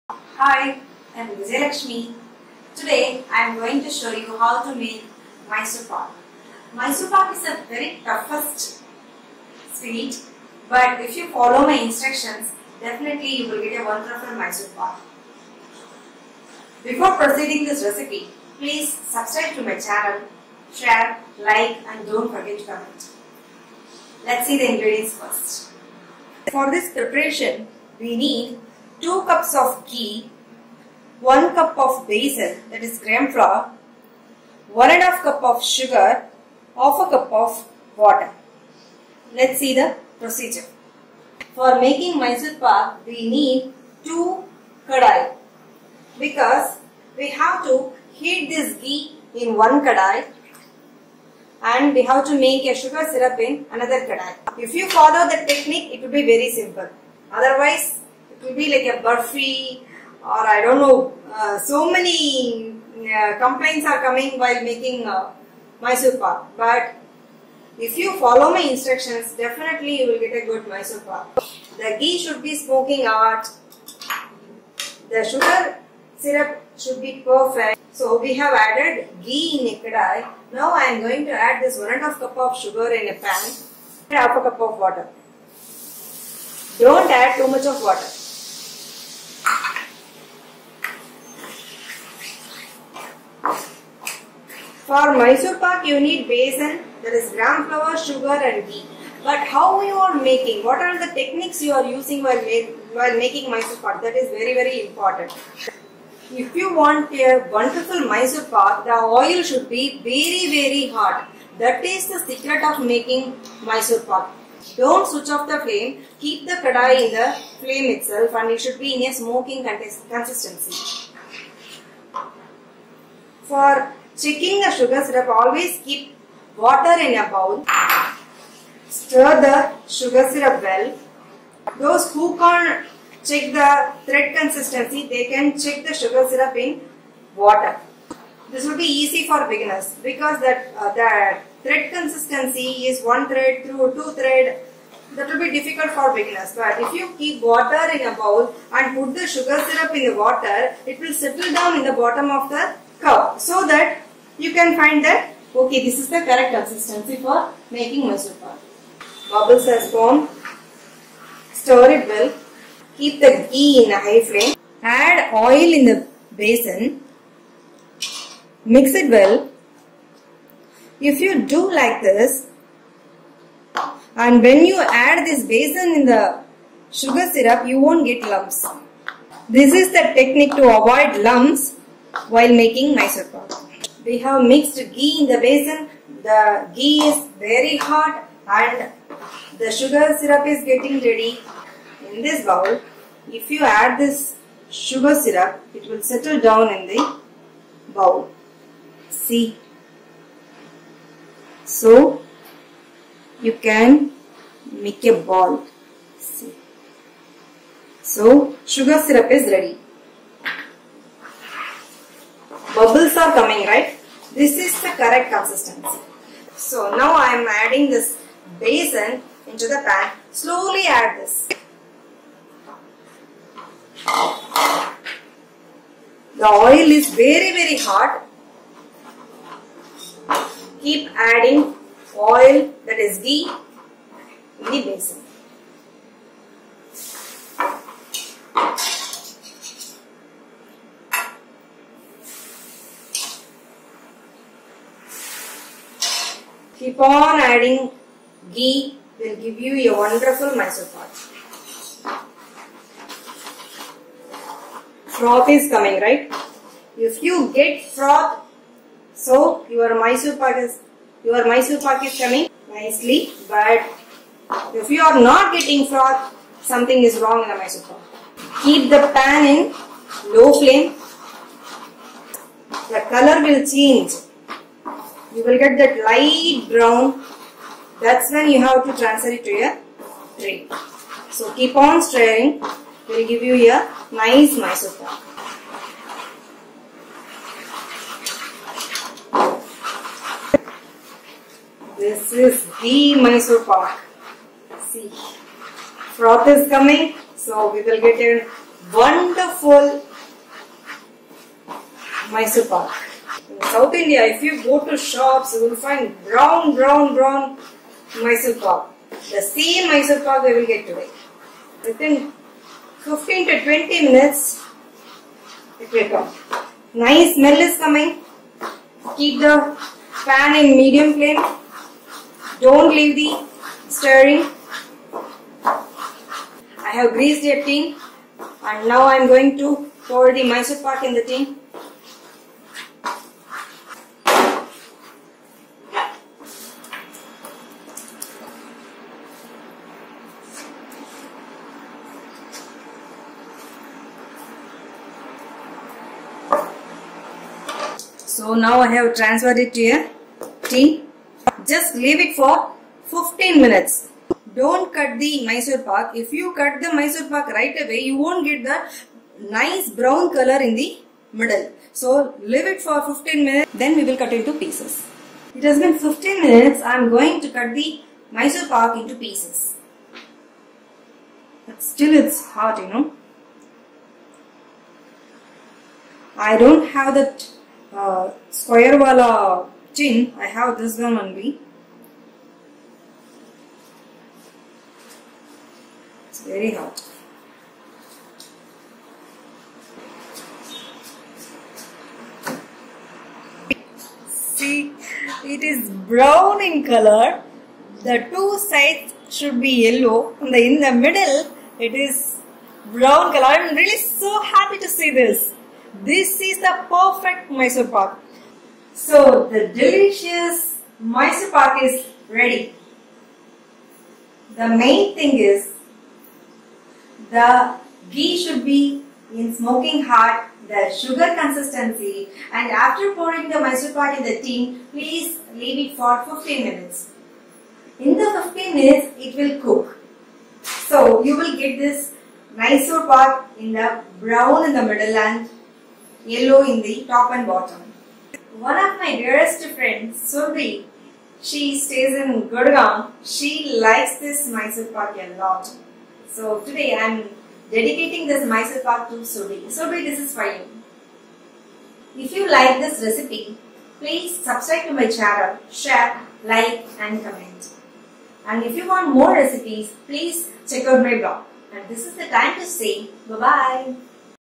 Perfect Mysore Pak Recipe | Make Mysore Pak Like its Made in South India
Mysore Pak is a famous sweet dish in South India. It tastes delicious when made in the correct way. This video will show you how to make Mysore Pak. Watch this video till the end to know the key points to make Mysore Pak perfectly. Perfect Mysore Pak is brown in the middle with lots of air bubbles and light yellow in colour on top and bottom.